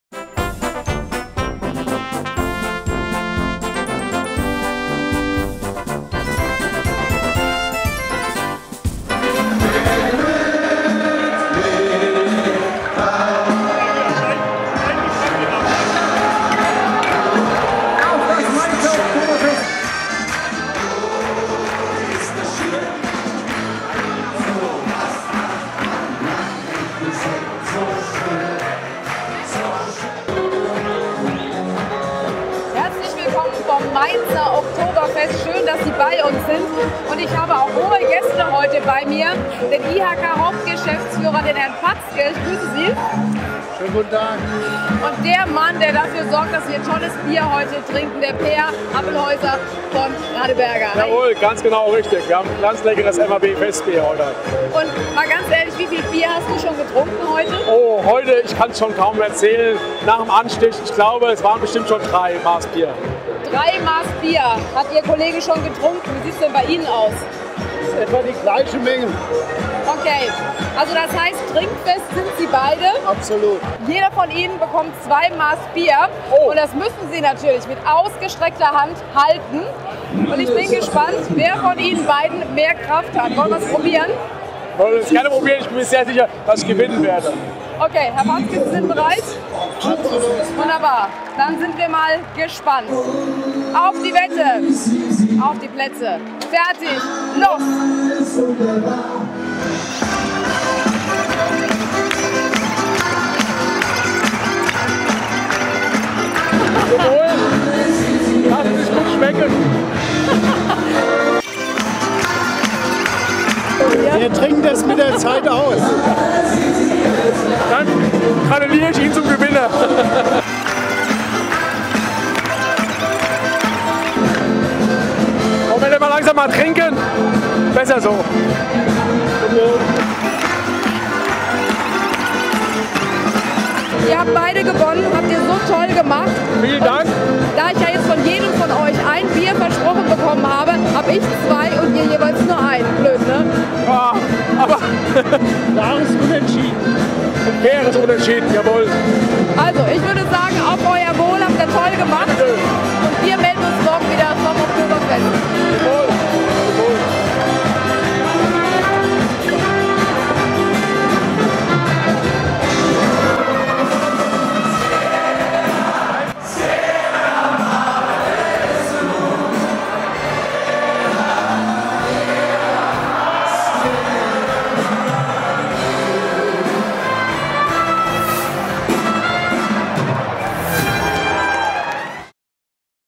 Meenzer Gaudi: Wettbewerb im Maßkrugstemmen
Radeberger gegen IHK - wer schafft es länger die zwei Maßkrüge zu stemmen? Rocklandradio Moderatorin Alexandra Philípps hat Richard Patzke von der IHK Rheinhessen und Peer Appelshäuser von Radeberger gegeneinander antreten lassen.